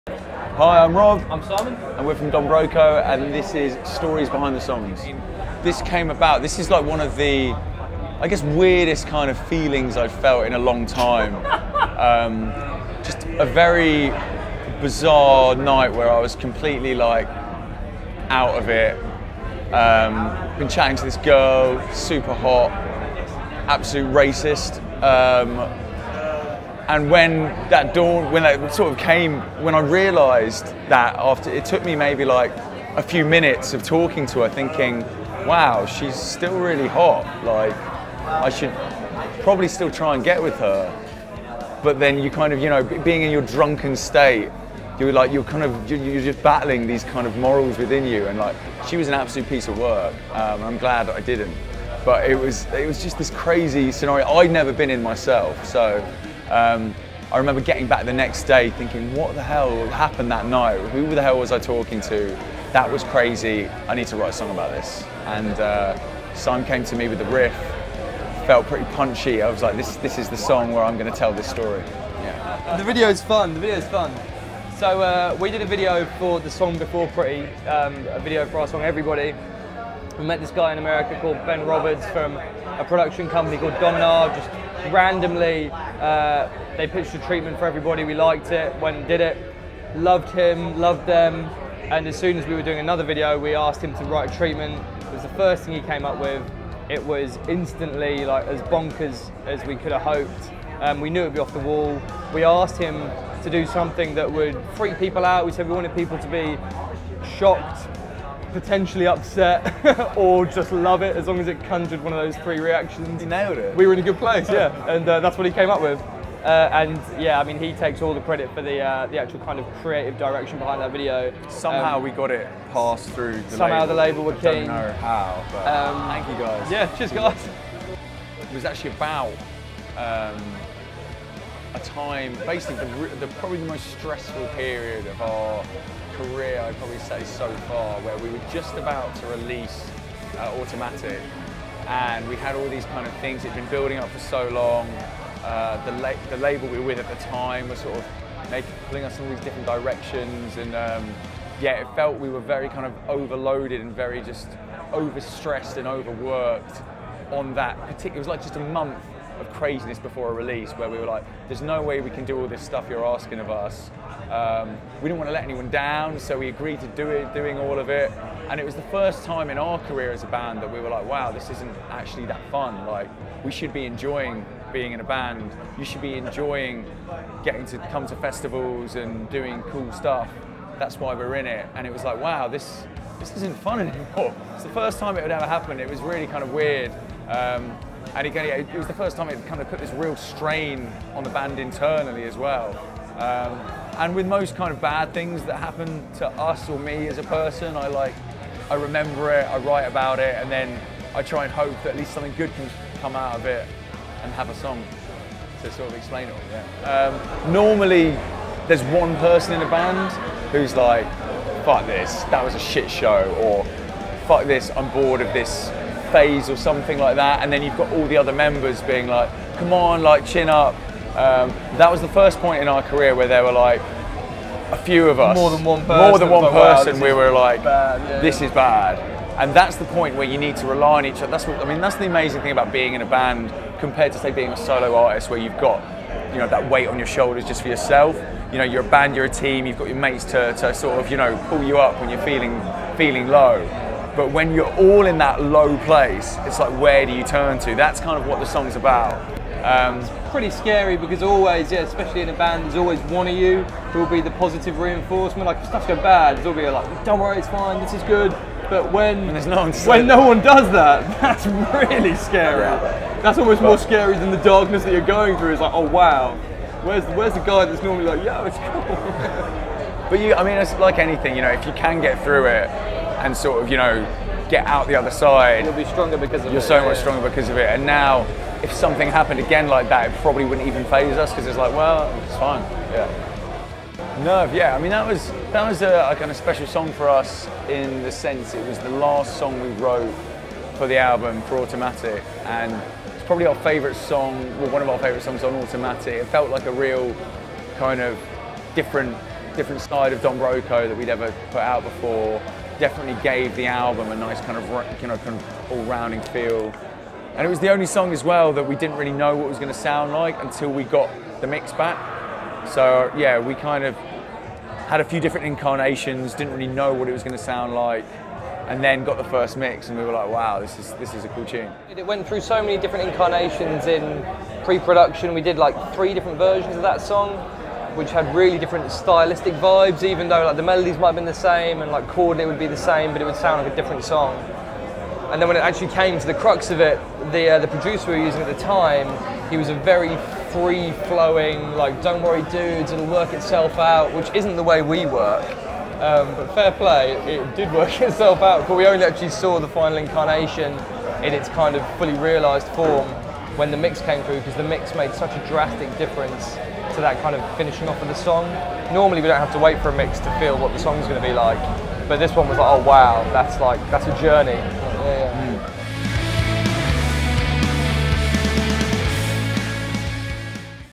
Don Broco - The Stories Behind The Songs
Don Broco's Rob Damiani and Si Delaney tell the stories behind their songs.